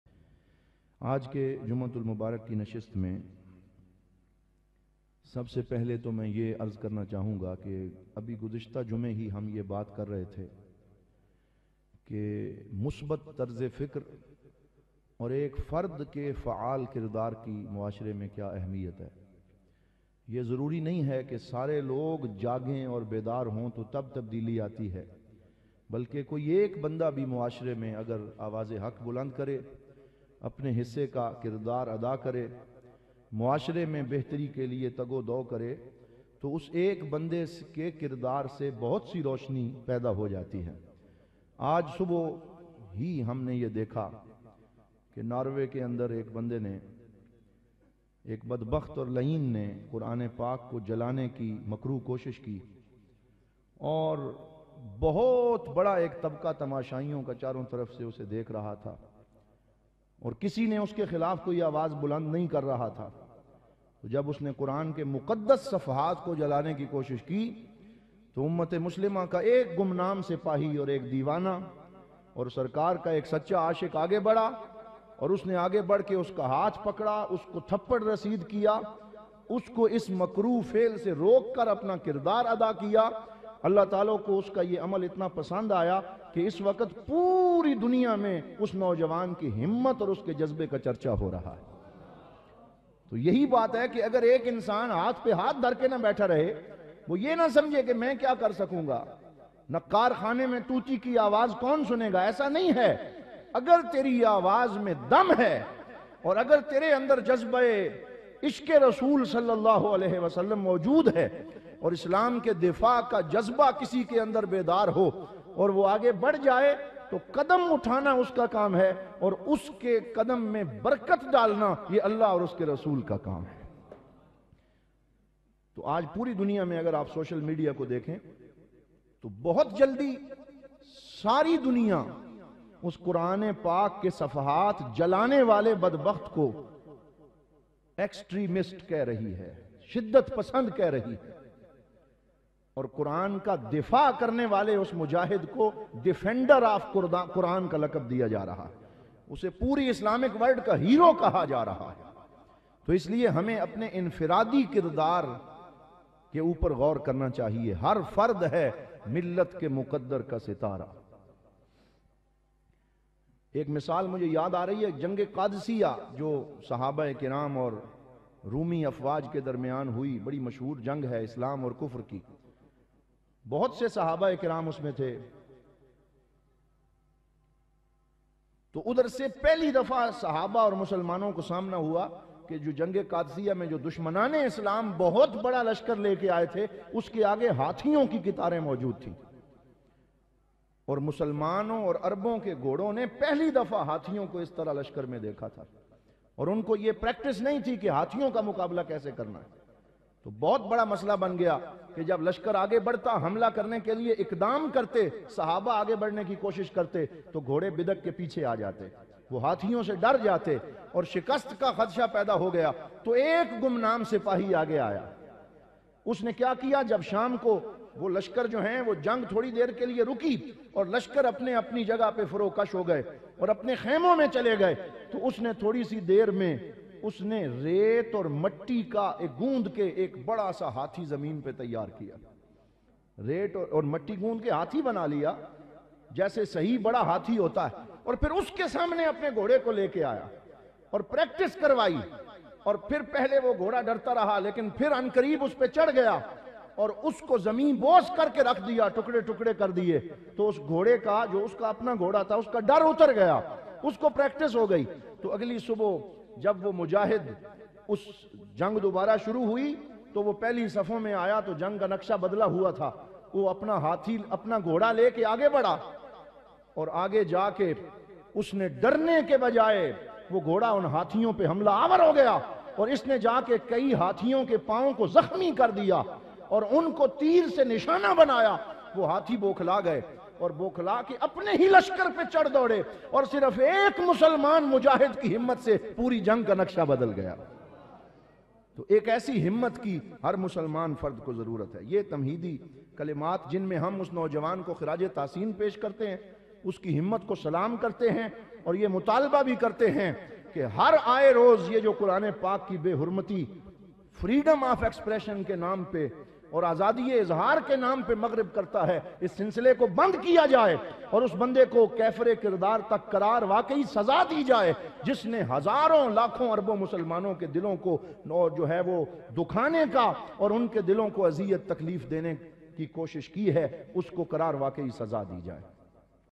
The defender of Quran e Pak in Norway by Dr Muhammad Naeem Ud Din Alazhari
The defender of Quran e Pak in Norway
Al Karam Friends International
The Official Organization of Astan e Alia Bhera Sharif and Darulaloom Muhammadia Ghousia Bhera Sharif.
office Contact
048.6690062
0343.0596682